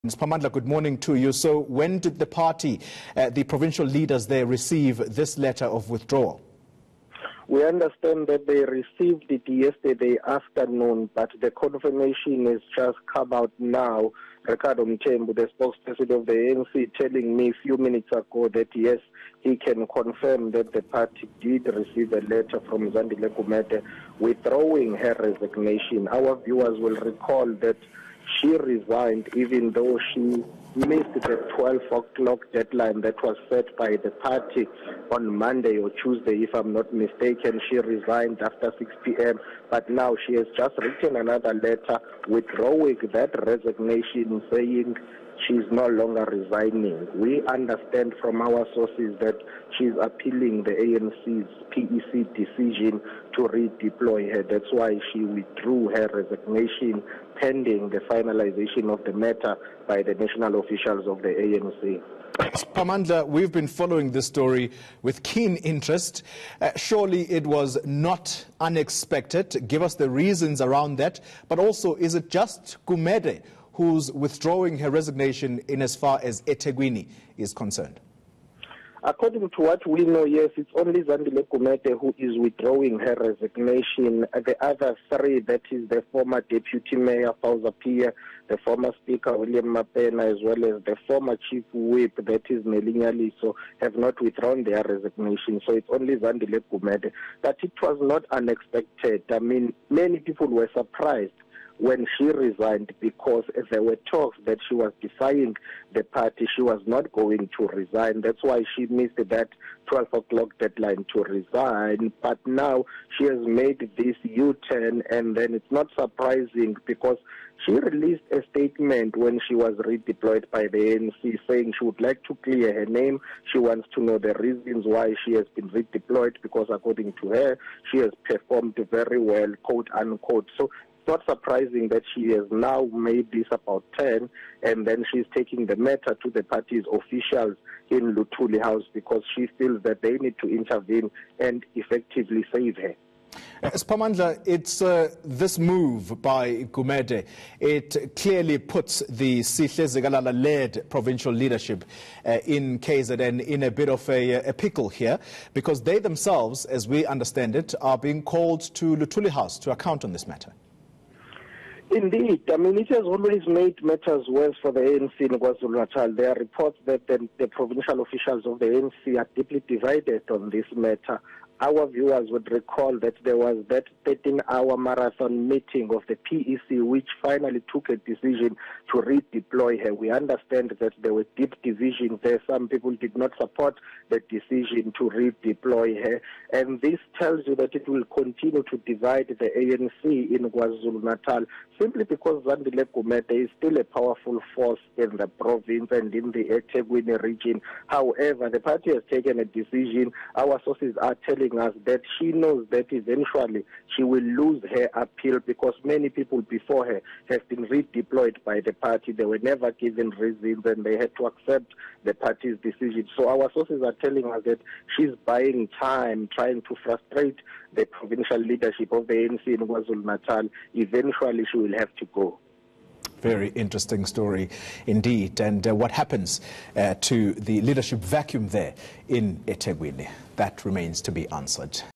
Gumede's u-turn
The ANC in KwaZulu-Natal says they've received a letter from Zandile Gumede confirming the withdrawal of her resignation. Courtesy